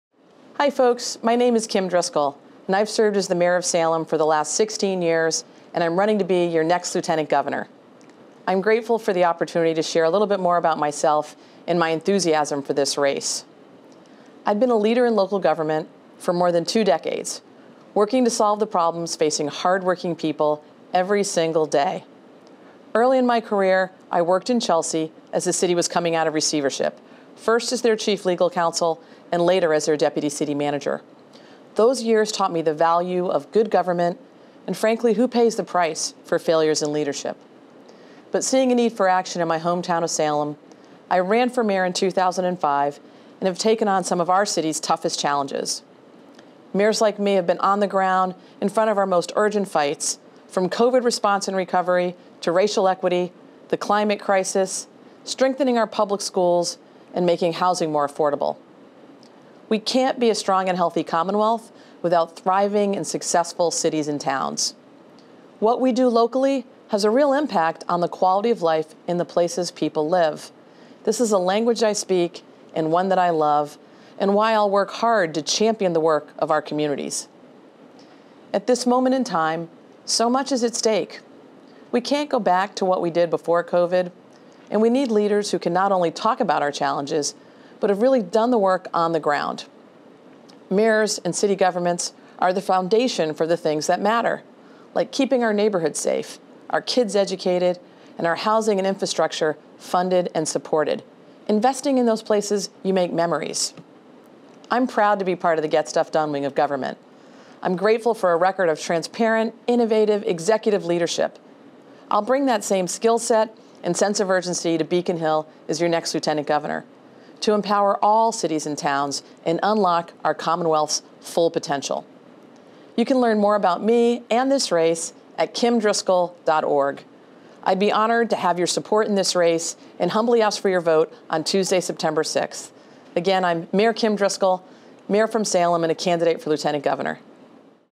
September 2022 Candidates Messages: Kim Driscoll
Kim Driscoll, Democratic Candidate for Lieutenant Governor, addresses Quincy’s voters for the September 2022 Primary Election.